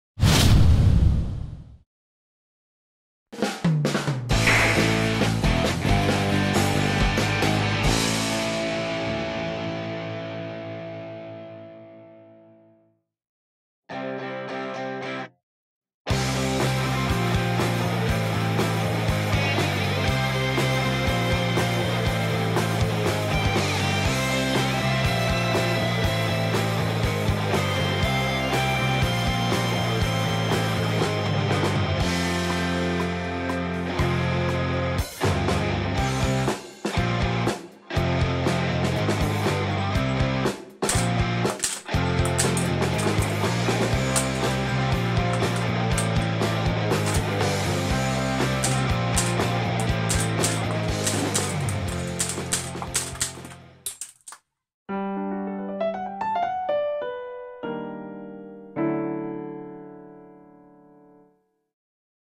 【モデルガン】Japanese modelgun Prop TANIO-KOBA/タニオ・コバ M4 I0連マガジンde10連射
10連マガジンでただ10発撃つ動画
Custom.556